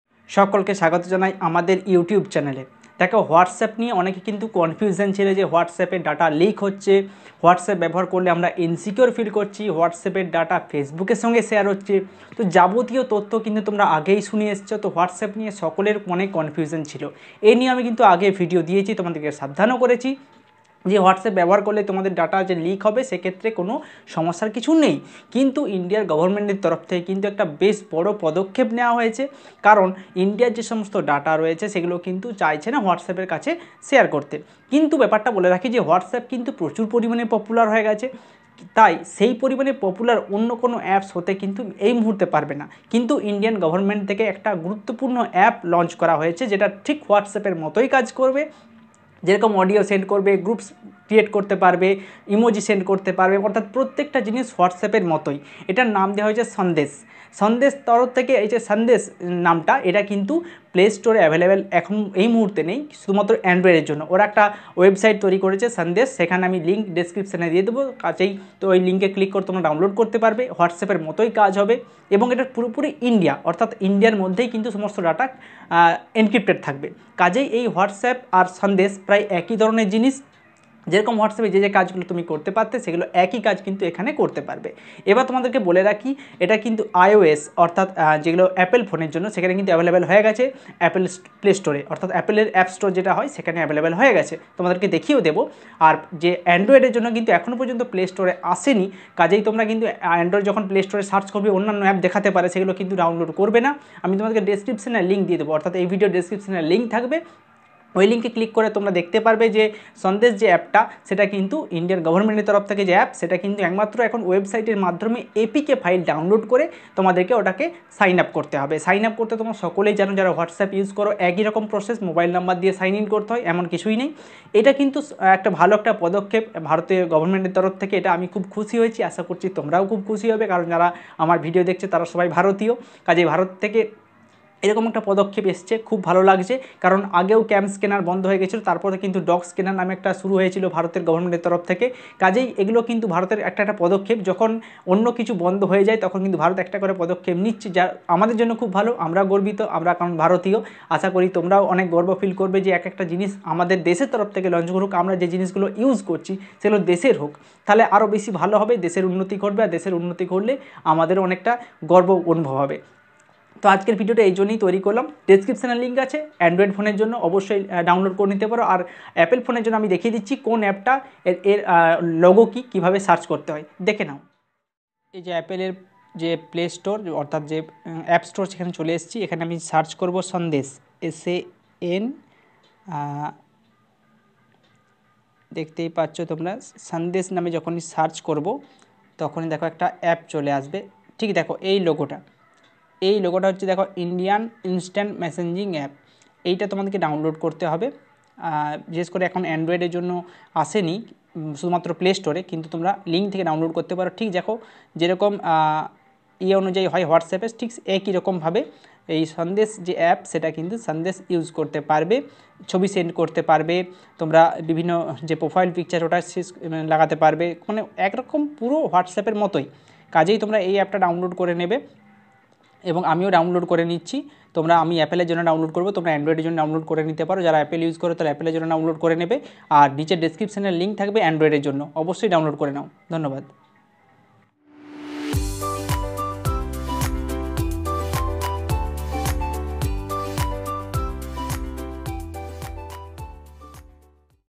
Sandes App : Indian Government || Whatsappএর বিকল্প 🔥 How to Download & use ??
ভিডিও সম্পুর্ণ দেখো এবং তোমার প্রিয় বন্ধু/বান্ধবী/আত্মীয় স্বজনদের কাছে শেয়ার করে দেবে ।

👉ইনস্টাগ্রামে আমাকে ফলো করতে পারো
user name:- geographywithPranab

       ❤️ভিডিও দেখার জন্য অসংখ্য ধন্যবাদ ❤️